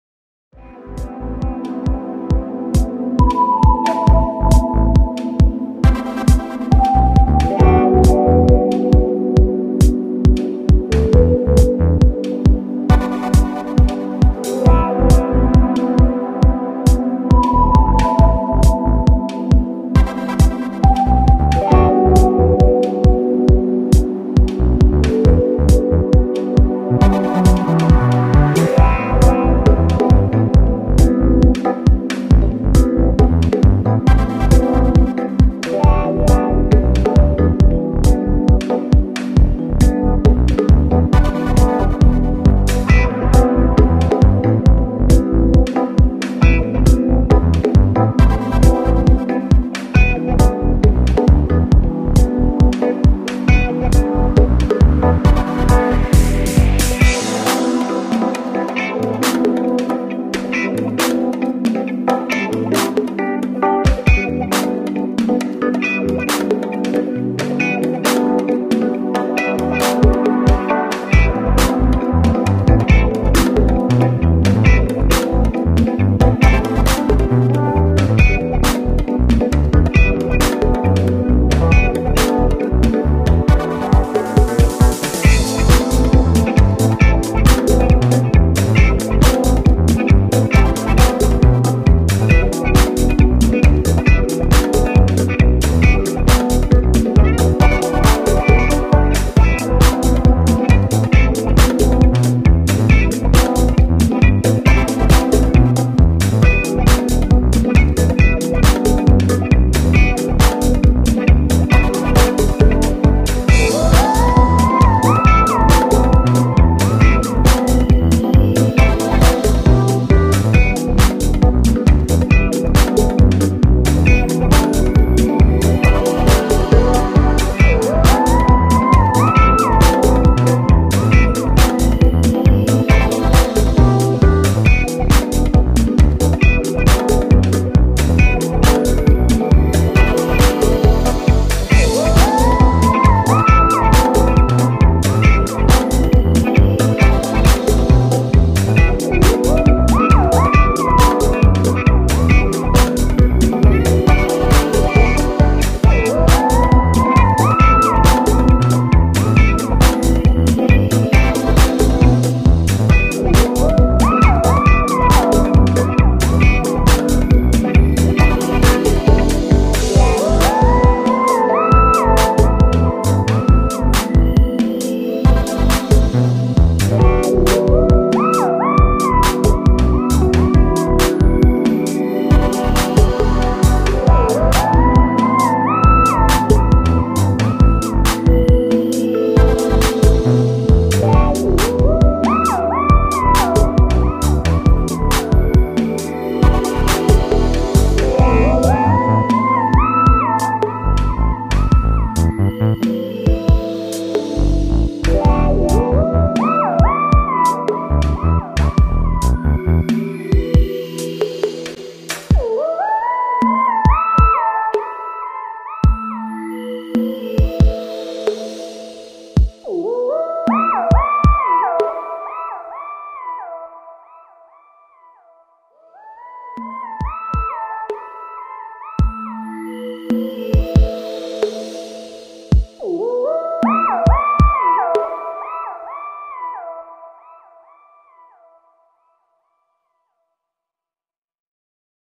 СЕРДЕЧКИ на зелёном фоне chromakey. СЕРДЕЧКО для видео монтажа green screen.
Сердце, сердечки chromakey для видео монтажа на зелёном фоне. Видео green screen с лицензией Creative Commons.